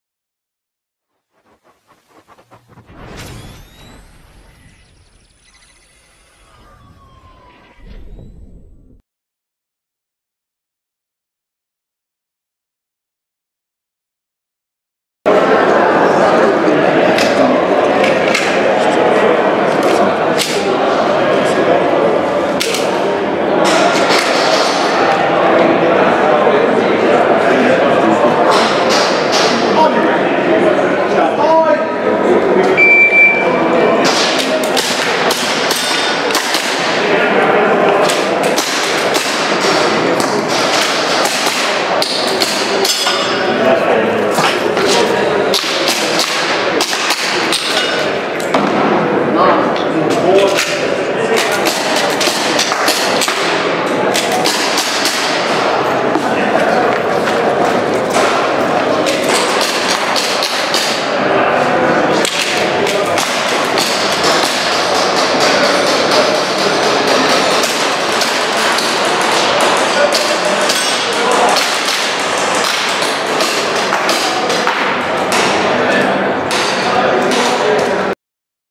Airsoft Surgeon European Championship 2016 Video 83
Shooter goes through a Course of Fire during the Airsoft Surgeon Practical Pistol European Championship 2016 that took place last 13-14 August at the StrikeForce CQB in Gloucester, United Kingdom.